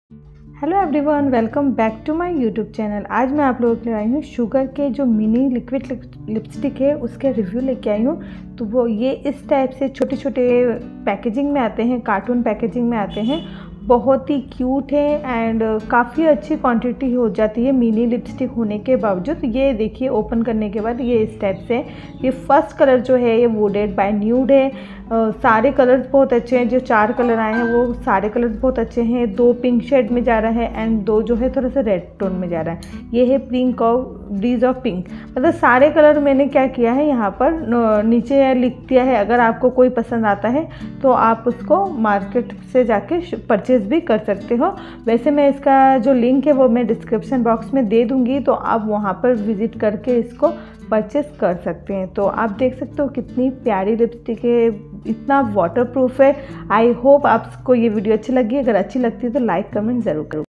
Sugar Cosmetic Liquid Mini Lipsticks Review and Swatches ll Sugar Smudge Me Not Liquid Lipstick
My channel name is Exotic Beauty Zone

product use:-
SUGAR Cosmetics Smudge Me Not Liquid Mini Lipstick Set

mobile use
Realme 9 pro plus

mobile use Samsung Galaxy a7

Ring light
Digitek LED RING LIGHT MODEL NO DRL 14C